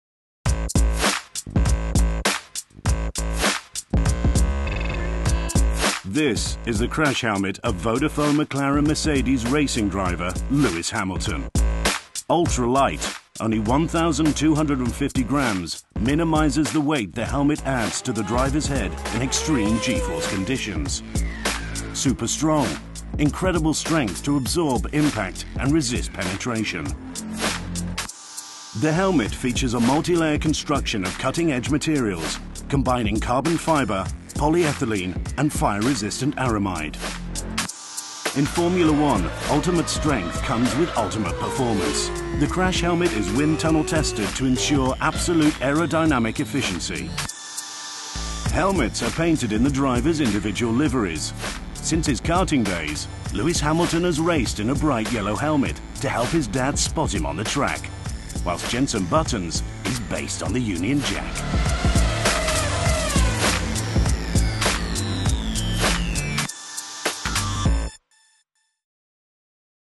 On Track With SAP - The Crash Helmet
On Track With SAP - a series of one minute films providing bite-size insights into Formula One technology. This clip focuses on the crash helmet.